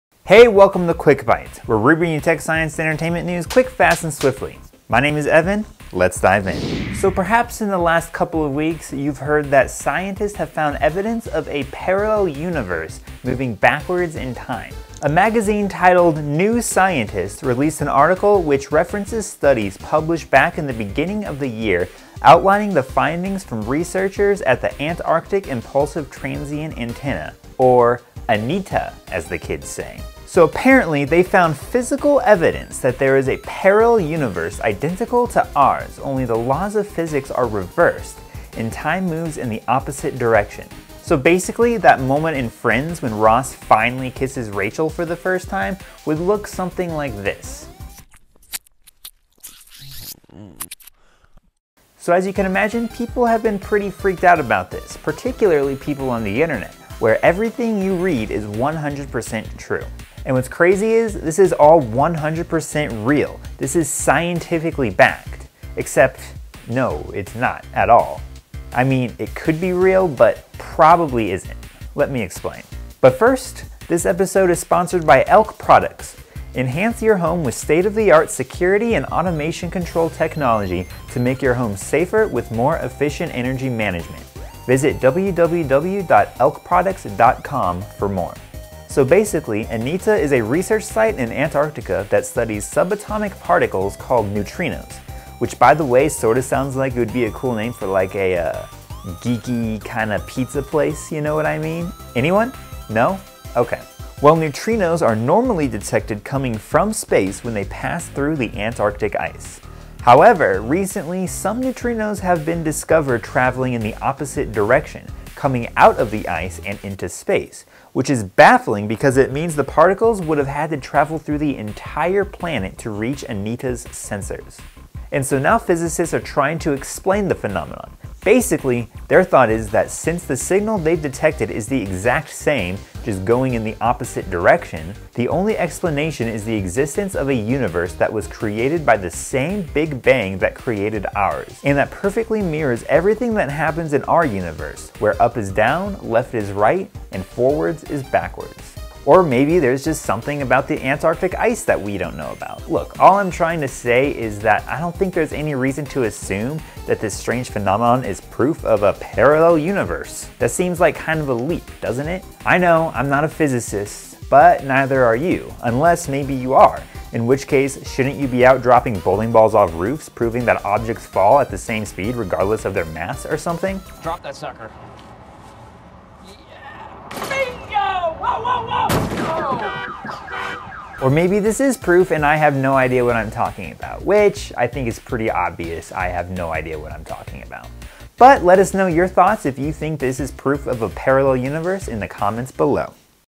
Did Scientists Actually Find Proof of a Parallel Universe? - Quick Bytes
People have been talking about how scientists may have found evidence of a parallel universe where the laws of physics are reversed and time moves in the opposite direction. But, should these new findings actually qualify as proof? Find out in this episode of Quick Bytes.

Also, make sure to follow our other social media platforms to stay up-to-date on all things Innovation & Tech Today.

Video : Evan Kelley, Dalton Brown